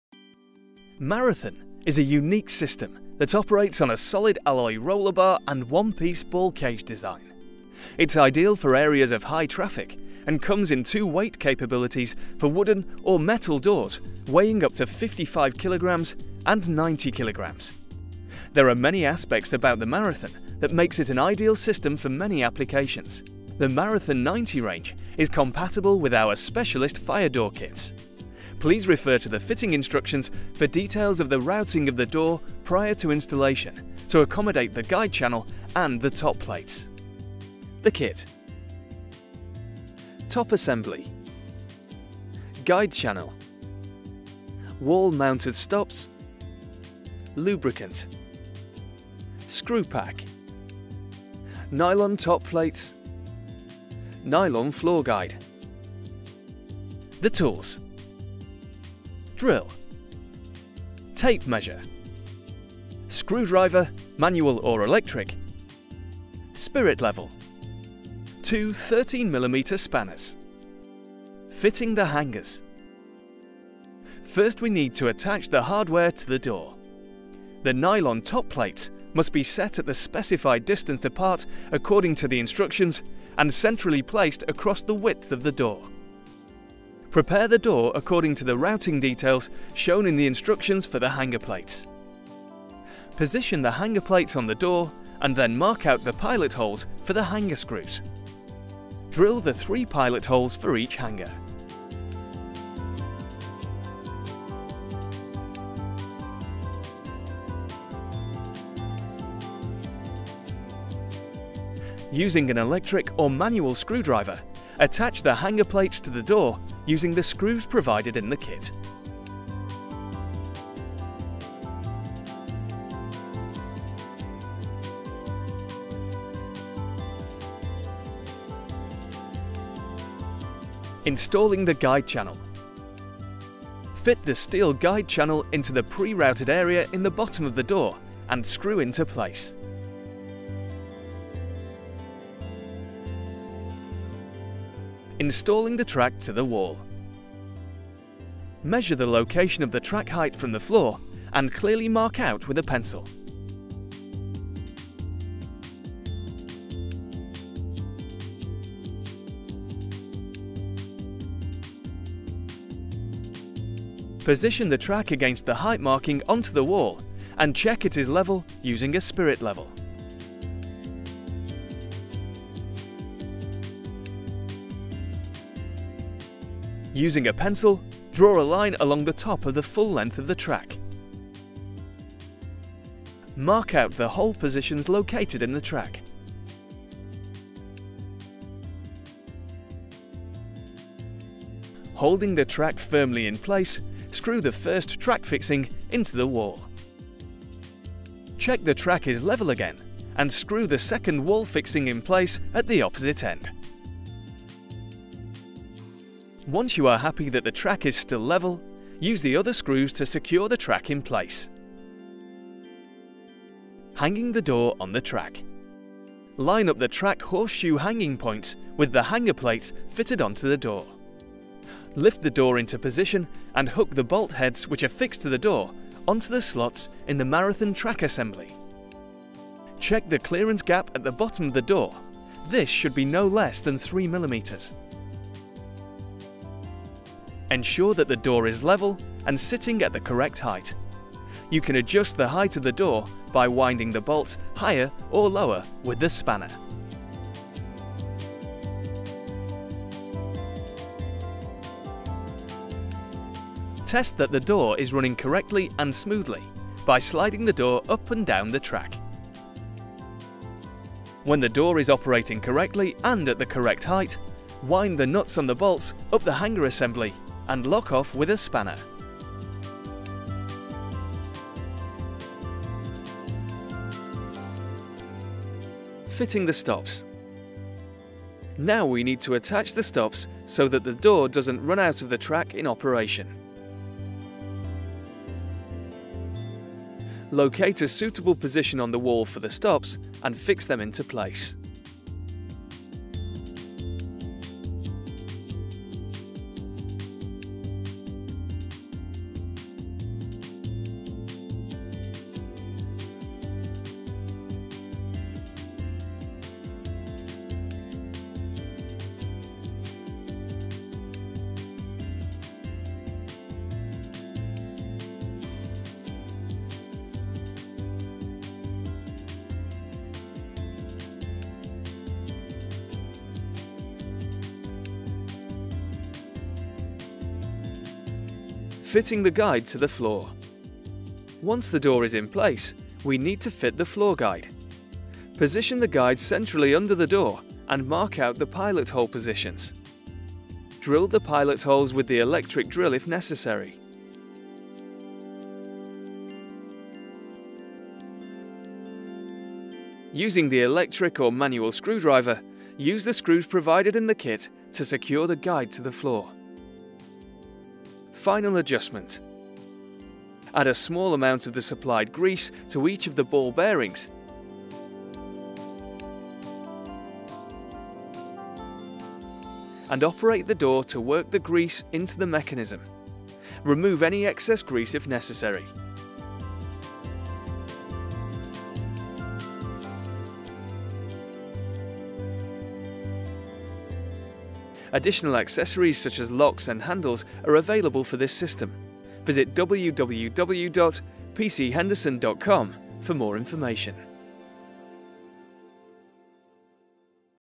HENDERSON MARATHON 1-DOOR SLIDING TRACK SYSTEM | Screwfix
Screwfix - Highly durable and smooth-running ball race runner bar system designed for internal doors and cupboards. Suits solid core doors in high use applications.